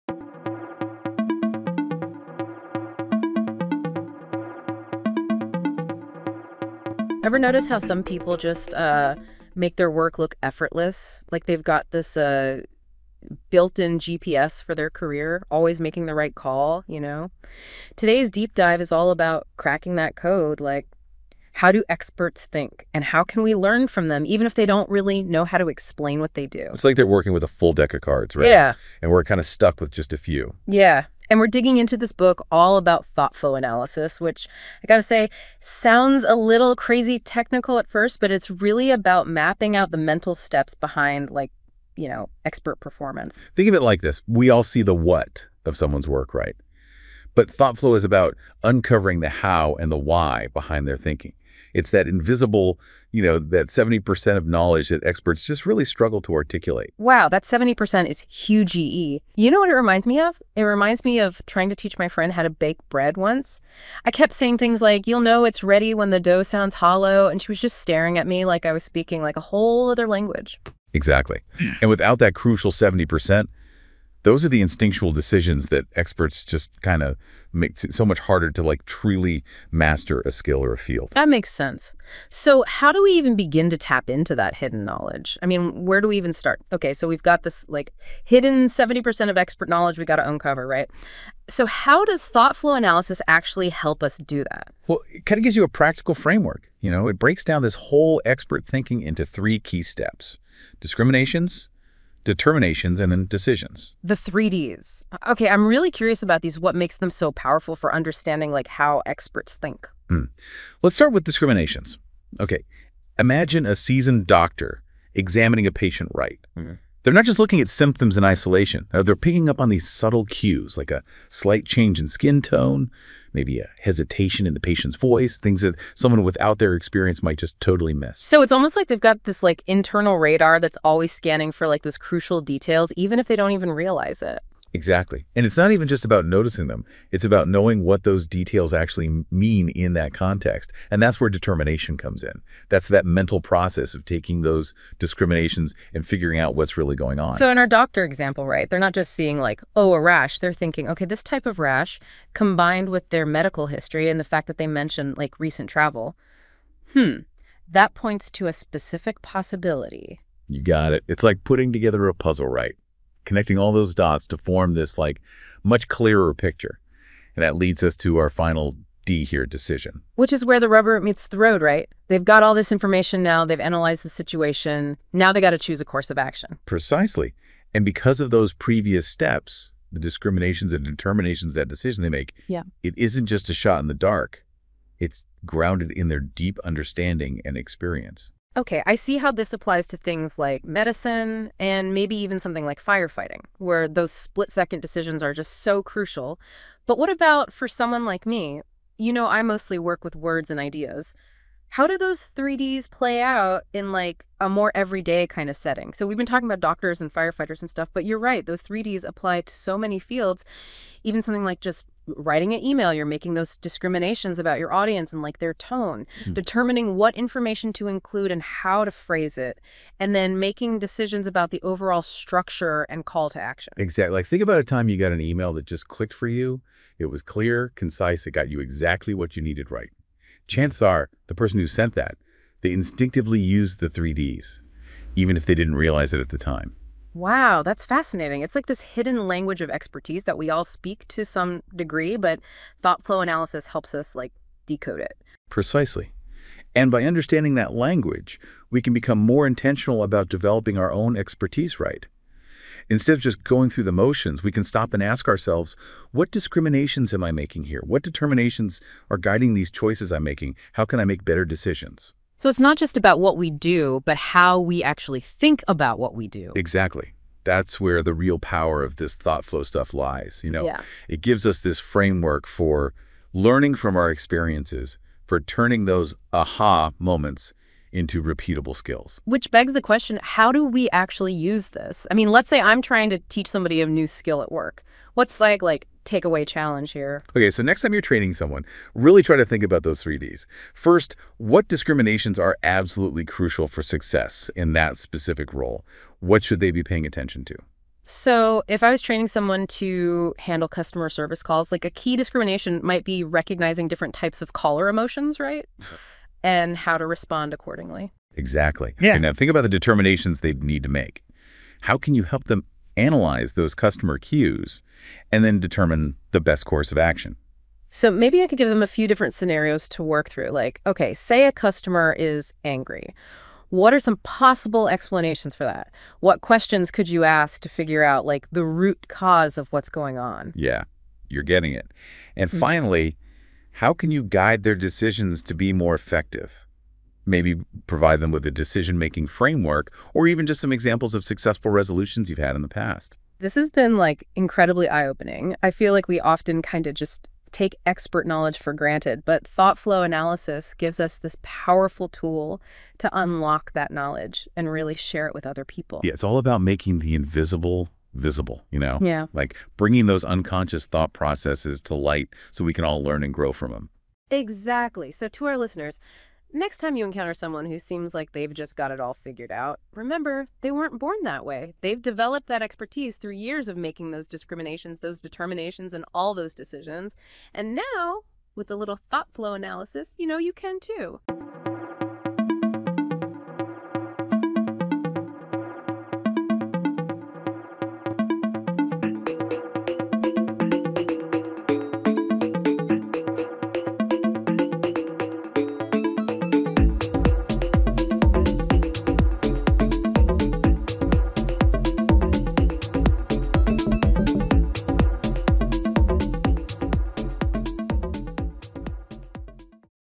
Notebook LM Podcast Book Review - The 3 Ds of ThoughtFlow Analysis 2021
The source is a book that provides a comprehensive guide to instructional development, focusing on the author's methodology, called Modular Curriculum Development (MCD). The book highlights the importance of "ThoughtFlow Analysis," a framework that identifies and analyzes the cognitive tasks learners perform alongside overt behavioral tasks.

The book systematically explains MCD's various phases, including Analysis, Design, Development, Pilot Testing, and Revision & Release, with detailed tasks and procedures for each phase. It aims to help instructional developers design and create effective training programs that address both overt behavioral tasks and the underlying cognitive processes.

Resources

The Book
- The 3 Ds of ThoughtFlow Analysis